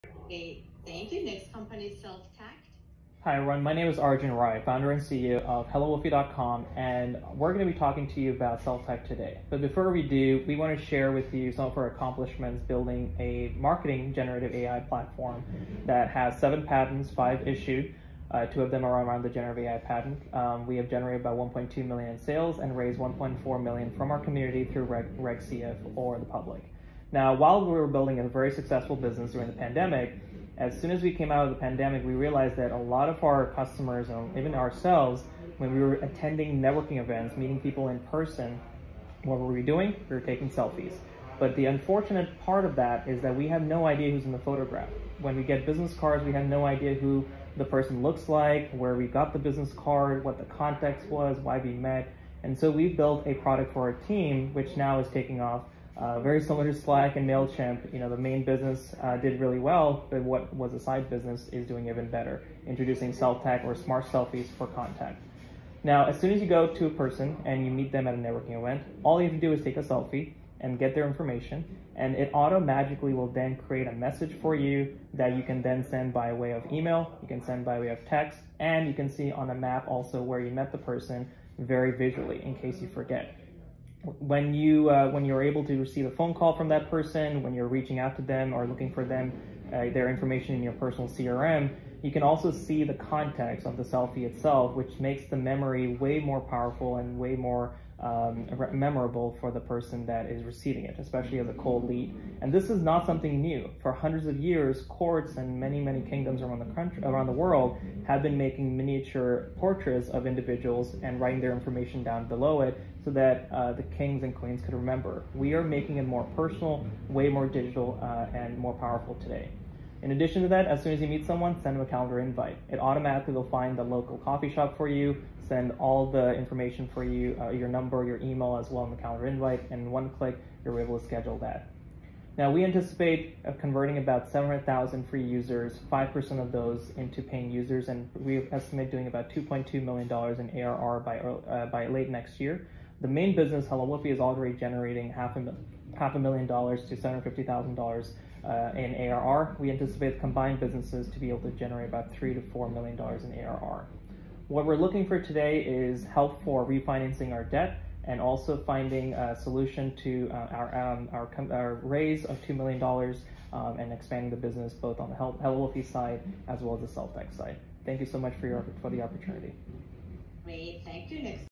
Pitching SelfTact and HelloWoofy at Plug n Play 🫡
Hey everyone, we just pitched our company Woofy, Inc. behind HelloWoofy.com and SelfTact.com at the Plug n Play Commerce Pitch session. Love your feedback and thoughts! 🫡

🤳🏽 SelfTact.com: Smart Selfies for Networking
🧬 HelloWoofy.com: Smart Marketing for Underdogs